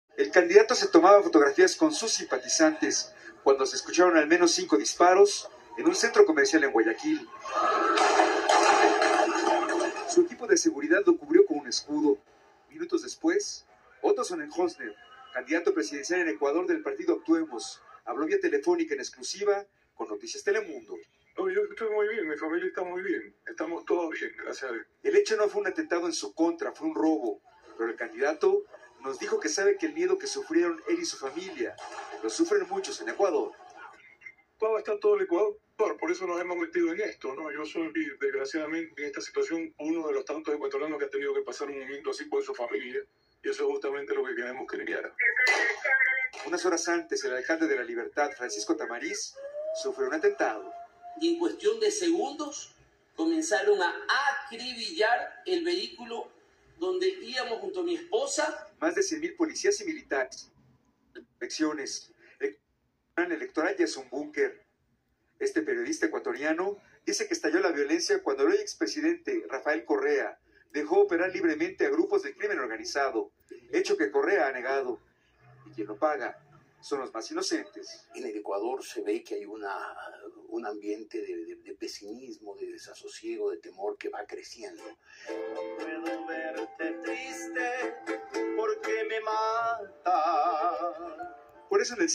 Sonnenholzner fue testigo de balacera en local.
Para Youtube:  no se puede decir más de la noticia, ella se explica por si sola.